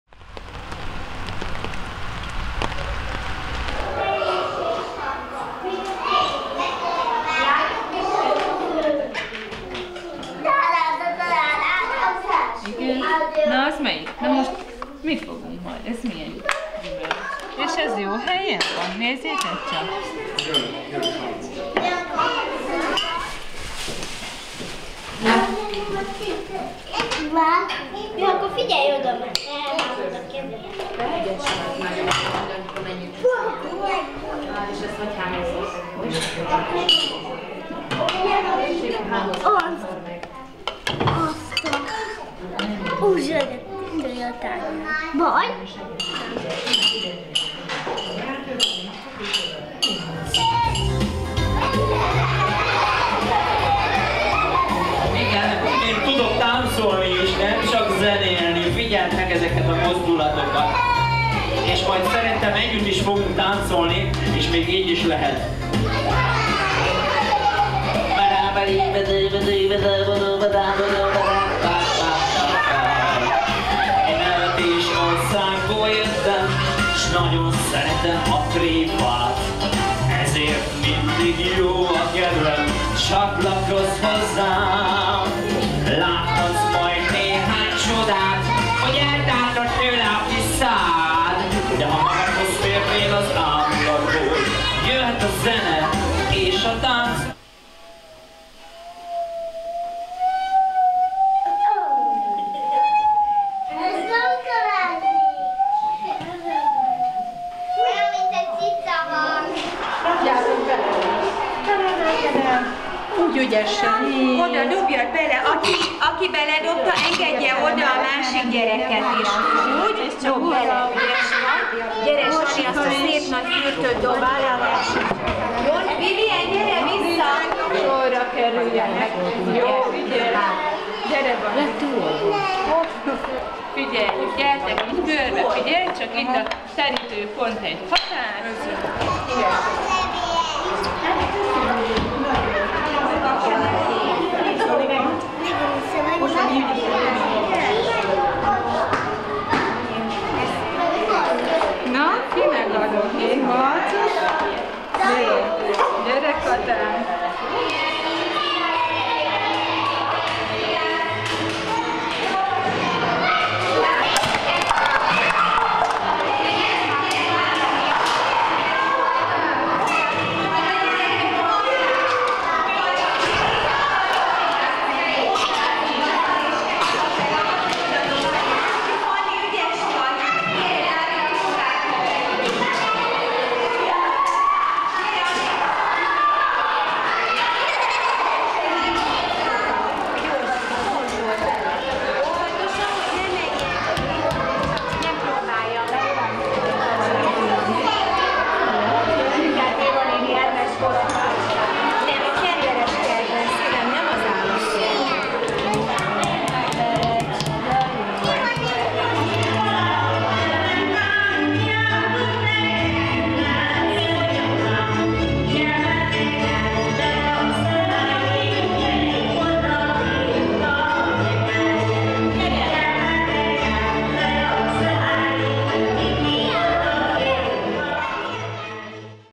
Hajdúnánási Televízió: Őszi játszónap a Gesztenyevirág Óvodában 2015.10.12.
Kommentár nélkül!
Változatos programokkal várták a gyerekeket az óvónénik.